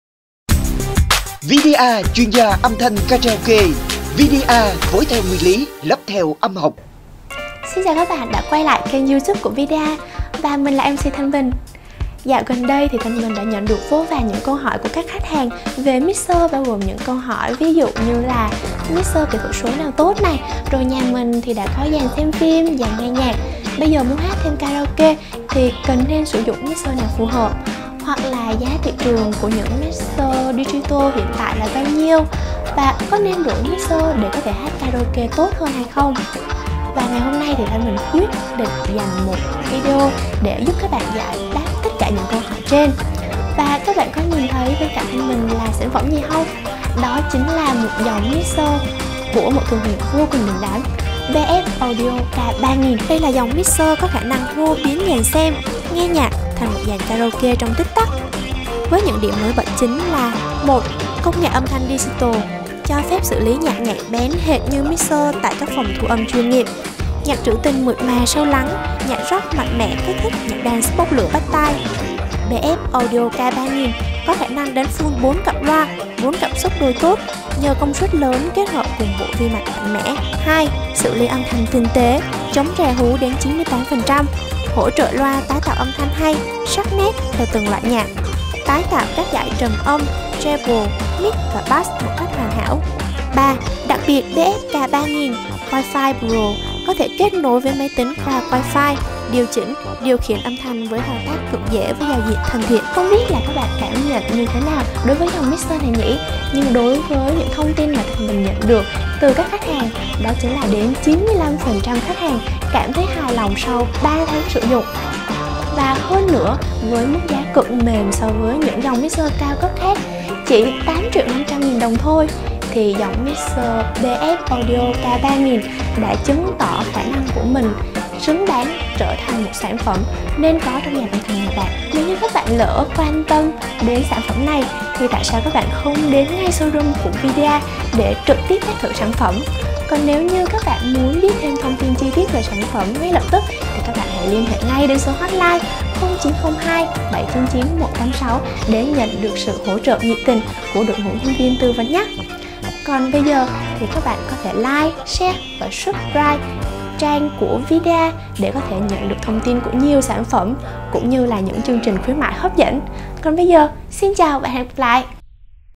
Hô biến dàn nghe nhạc, dàn xem phim thành dàn karaoke với mixer karaoke cao cấp BFAudio K3000WifiPro
207 Tân Phước, P.6, Q.10, TP.HCM